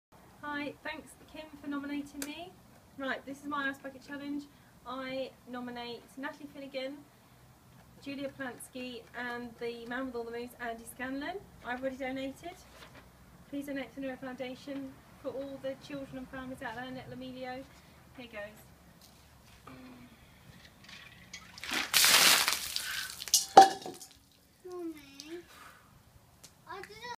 ICE BUCKET CHALLENGE FOR THE NEURO FOUNDATION
Please Text NEUR95 £3 to 70070 to donate £3 to the Neuro Foundation, who support families like ours to cope with Neurofibromatosis or NF1 for short.